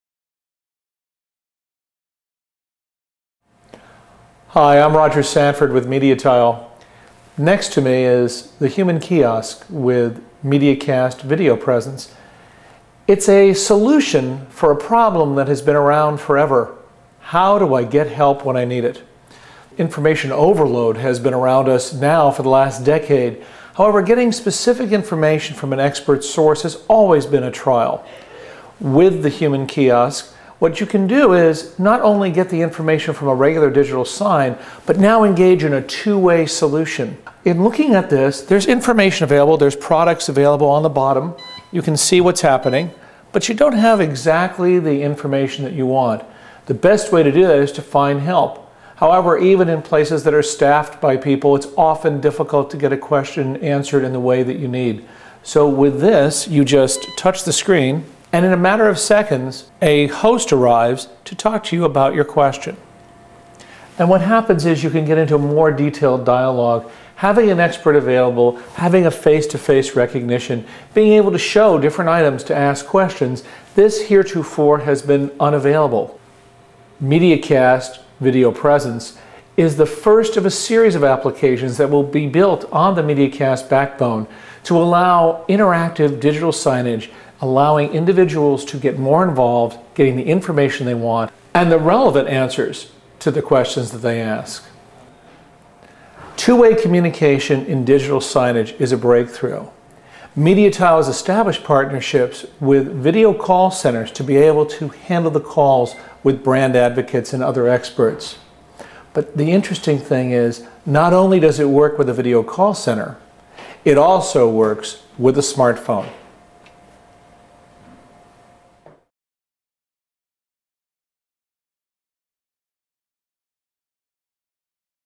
MediaTile HumanKiosk with 4G Video Presence
The HumanKiosk is a new product proof-of-concept that will revolutionize the brand-to-customer relationship. More than a digital sign, it enables brands to run digital promotions directly on in-store displays, and utilizes 4G technology to deliver a live, 2-way in-store video session between a consumer with a product question, and a representative of that product or service.